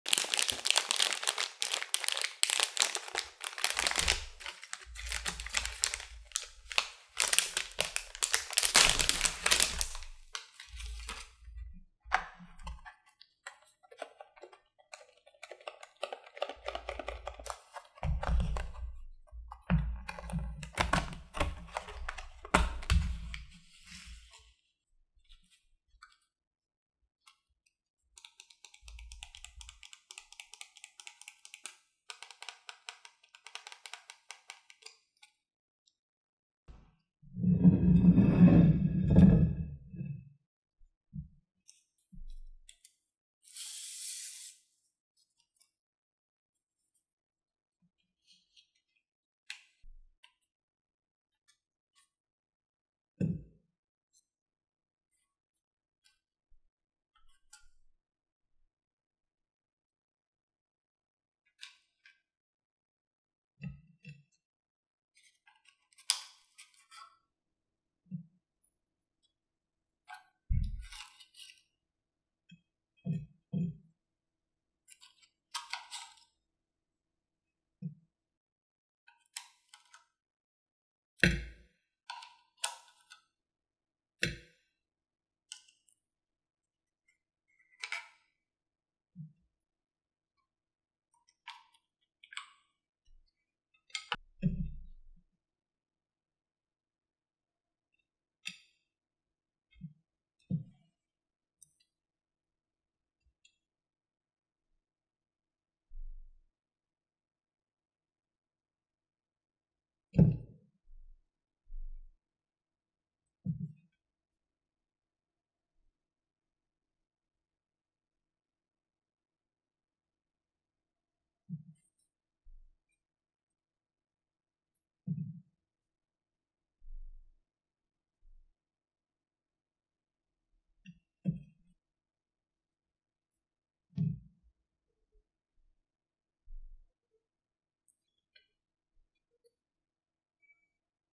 ASMR Unboxing Chupa Chups Mini Candy Sushi 🍣🍬
Welcome back, my lovely ASMR family! 💕 In today's video, I'll be unboxing and plating the delightful Chupa Chups Mini Candy Sushi 🍣🍬 Experience the relaxing and satisfying sounds as I unwrap and plate these adorable and delicious treats.

As always, this video is best enjoyed with headphones 🎧 for the full binaural experience. Sit back, relax, and let the sounds transport you to a world of sweet serenity. 🌟

🍬 Chupa Chups Mini Candy Sushi: These delightful candy sushi pieces are inspired by popular sushi dishes, including nigiri, maki, and uramaki, and made with Chupa Chups' signature fruity flavors. They make for a fun and tasty treat, perfect for sharing or indulging in by yourself!